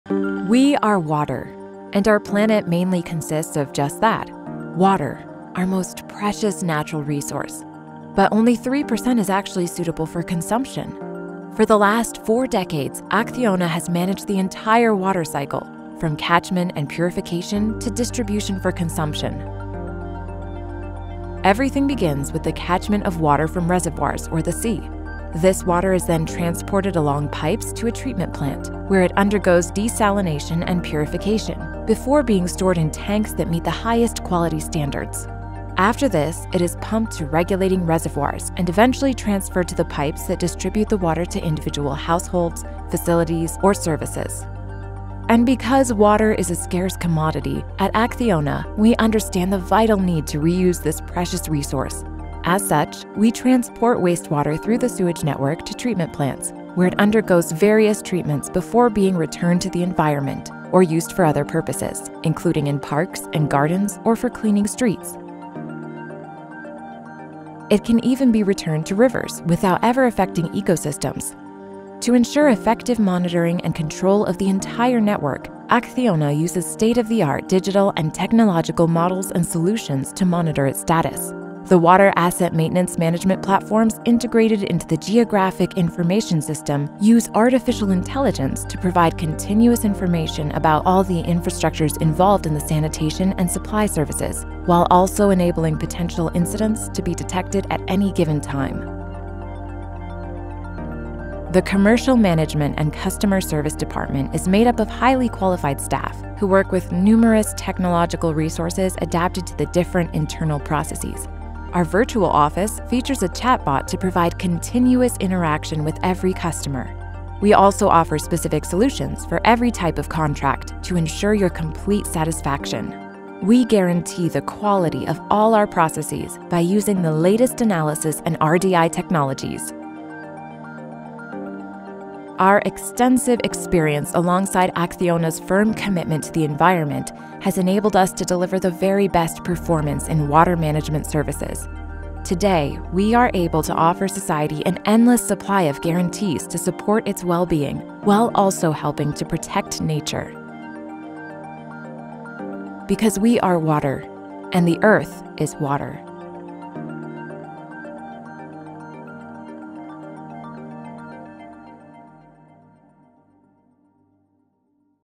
How do we manage the whole watercycle? | ACCIONA
We protect water at every stage of its life cycle to ensure the provision of quality water and its sustainable use. We are pursuing the goal of access for all to this essential commodity, as set out in the 2030 Agenda.

As a company, we have always had a sustainable approach to managing the water cycle comprehensively: we optimise the consumption of natural resources, minimise polluting resources in watertreatment processes, maintain management standards for waste water treatment, and undertake awareness campaigns for the end consumer.